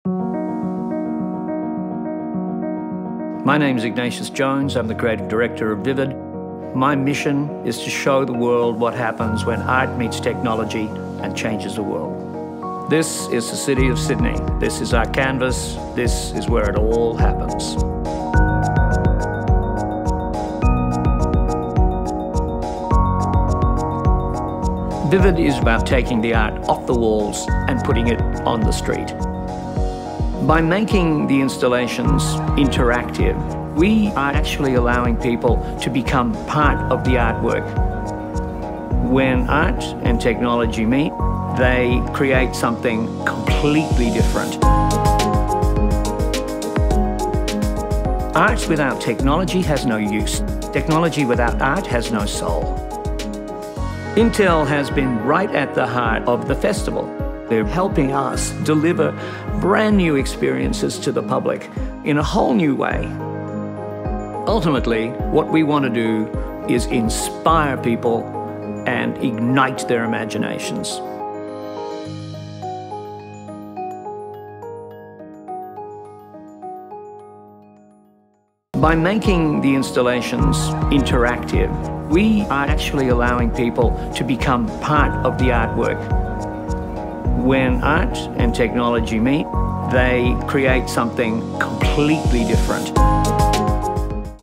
Experience Vivid Sydney | Intel
Anything is possible when art meets Intel technology. Watch how Vivid Sydney’s Creative Director transformed the city into a playground for drone symphonies, RealSense water shows, and projection mapped history on the iconic Opera House sails.